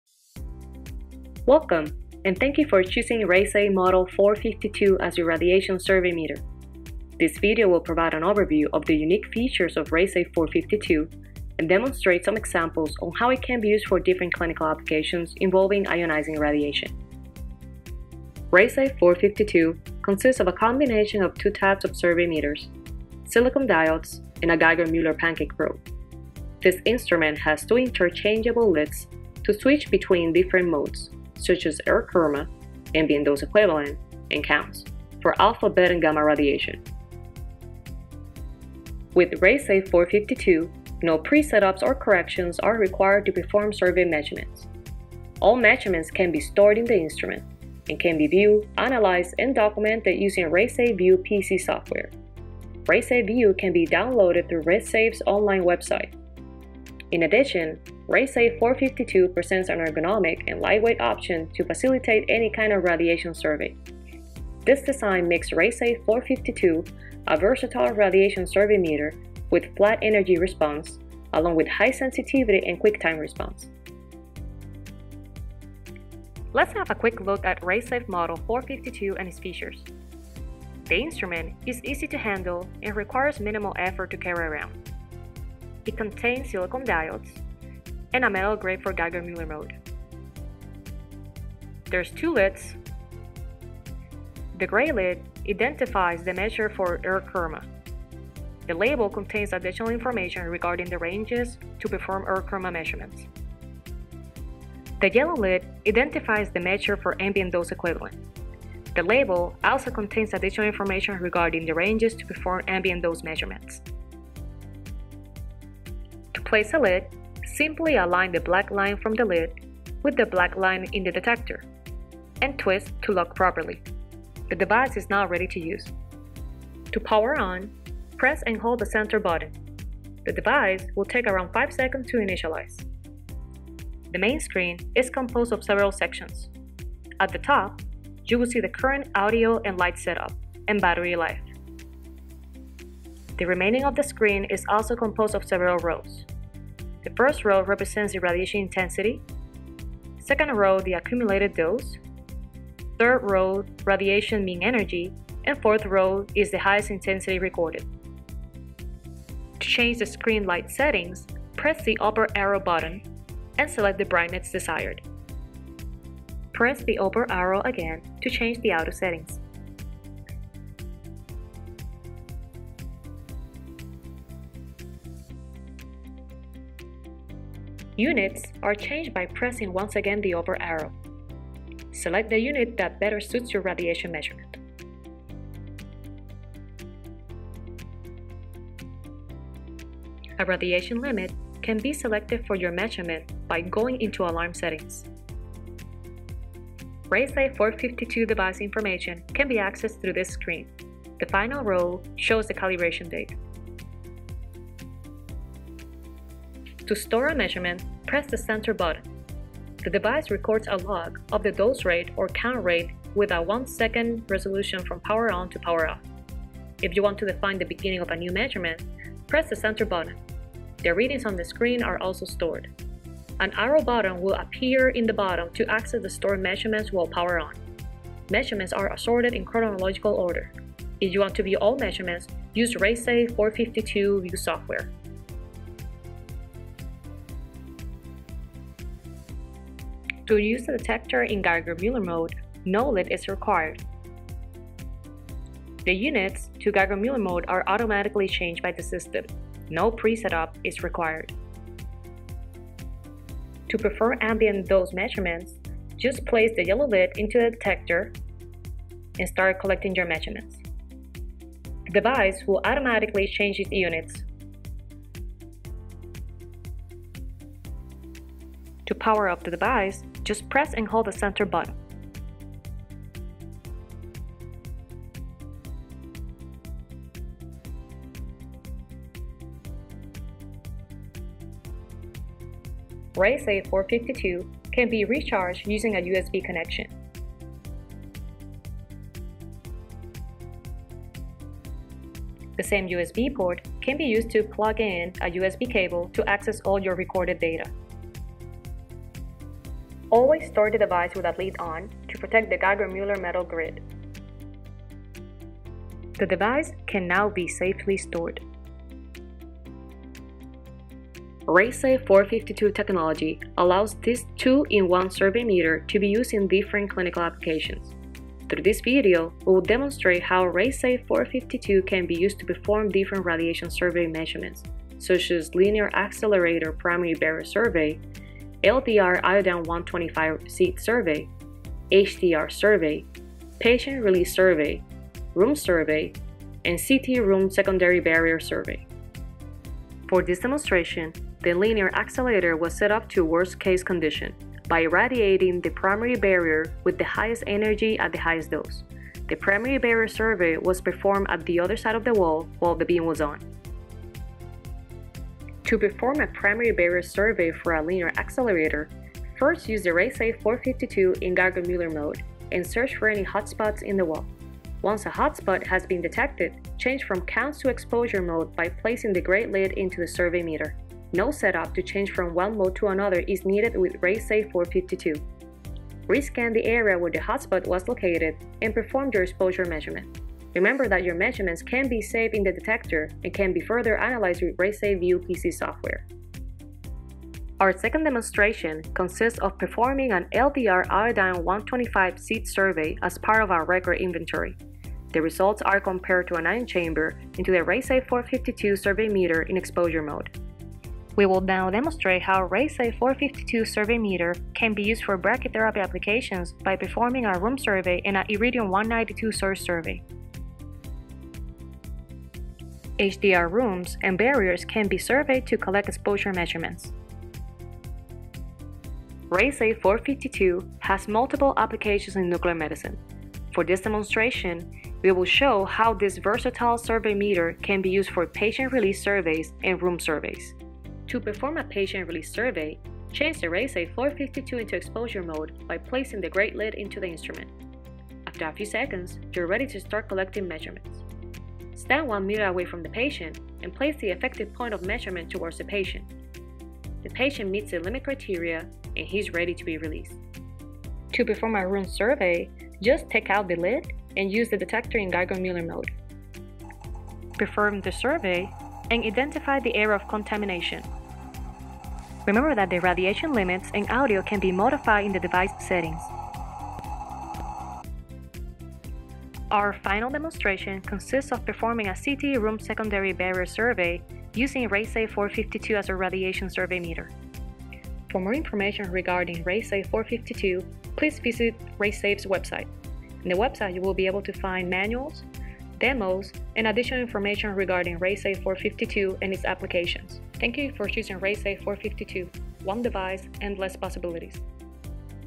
Clinical Applications of the RaySafe 452 Radiation Survey Meter
With an easy-to-use interface, multiple applications, and an ergonomic design, the RaySafe 452 Radiation Survey Meter is a top-of-the-line radiation measurement device. As you will see in this video, it is built for convenience and versatility.

The RaySafe 452 does not require any corrections or manual settings, letting you focus on radiation protection rather than setup. Just turn on the instrument, and within a few seconds, you are ready to measure.

The RaySafe 452 survey meter can be used for long hours due to its’ strong rechargeable battery. The brightness can be easily adjusted based on your preferences.